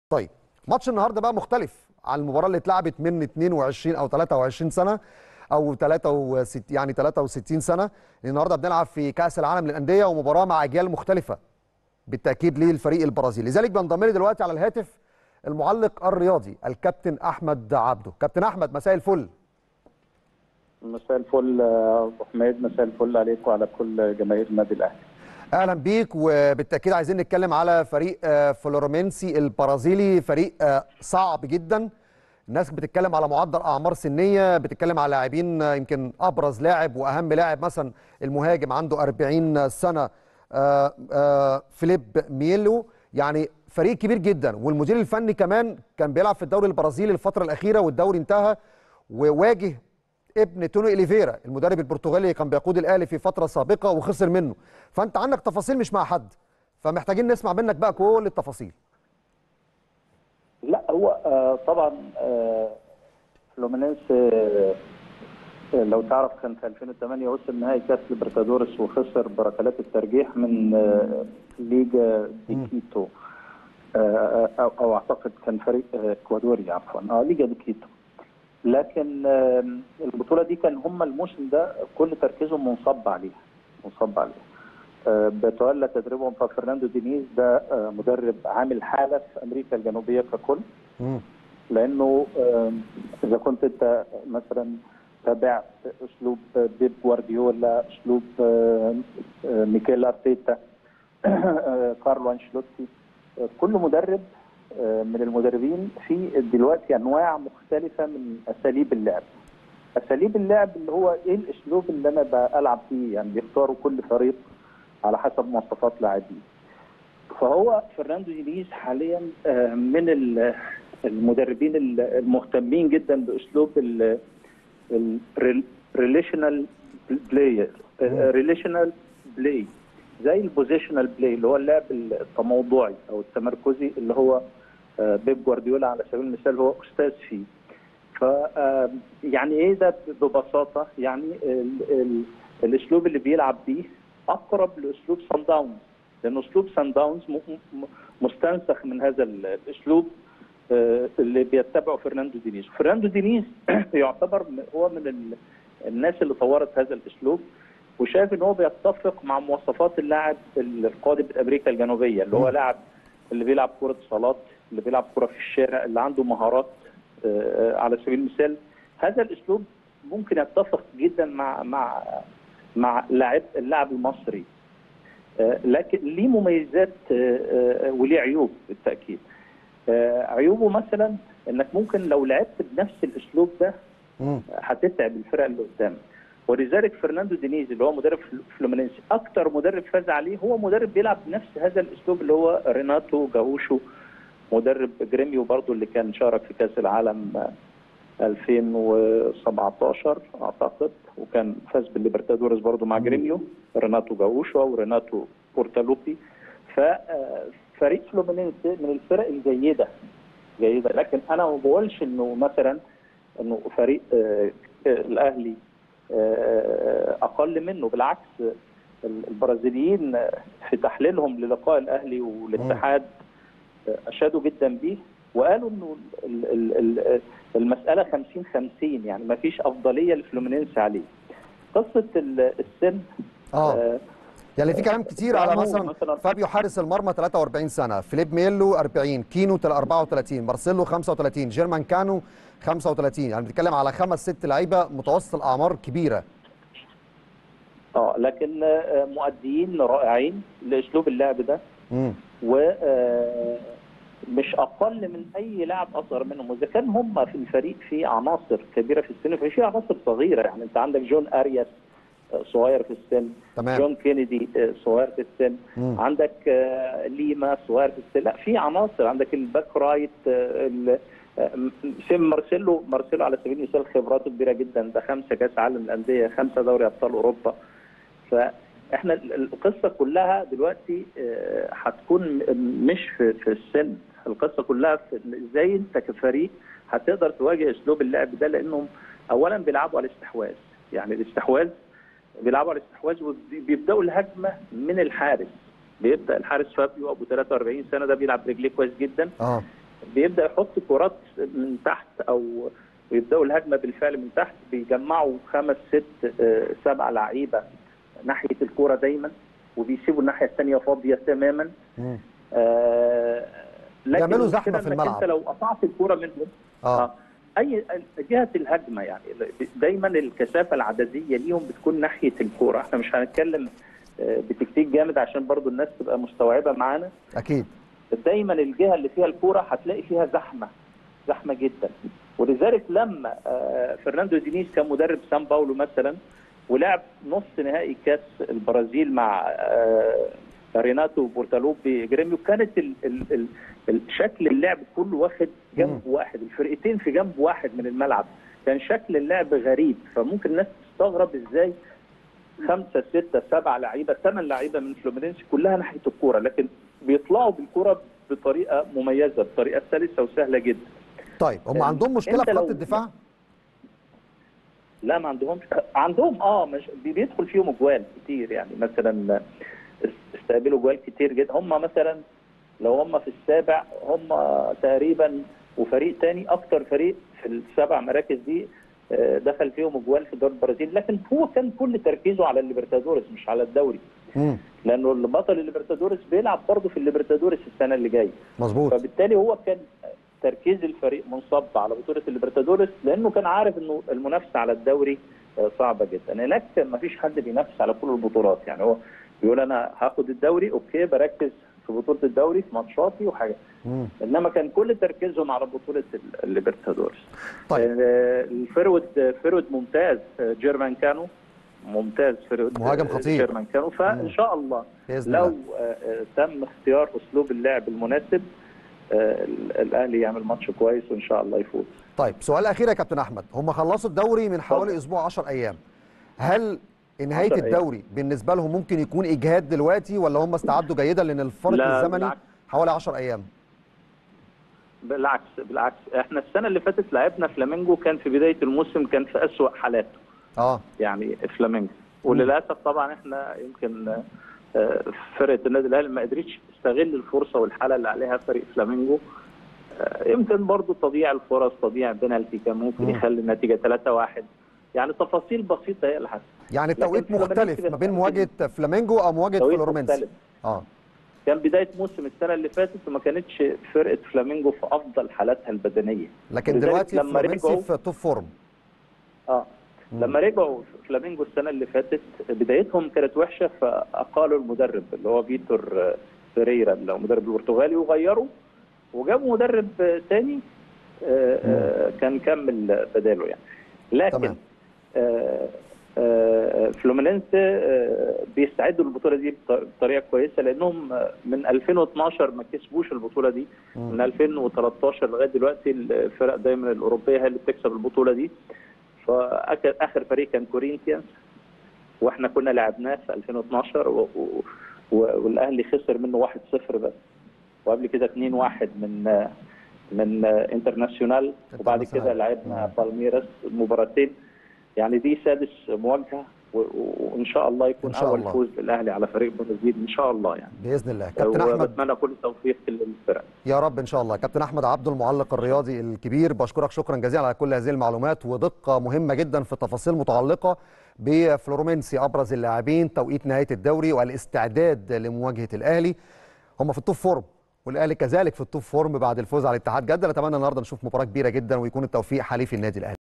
أحمد عبدة المعلق ورؤيته الفنية لمباراة الأهلي وفلومينينسي بكأس العالم وطريقة لعب الفريق البرازيلي
أحمد عبدة المعلق والمحلل الرياضي ورؤيته الفنية لمباراة الأهلي وفلومينينسي بكأس العالم للأندية وطريقة لعب الفريق البرازيلي